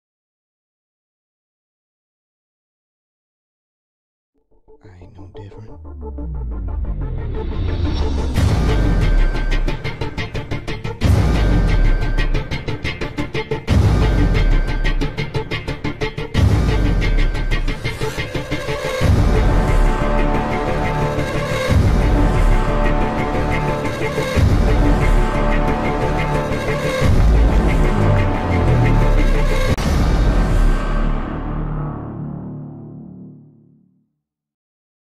The Adventures of Emile Heskey Pt9 - Trailer
A Trailer for the hotly anticipated ninth instalment of The Adventures of Emile Heskey.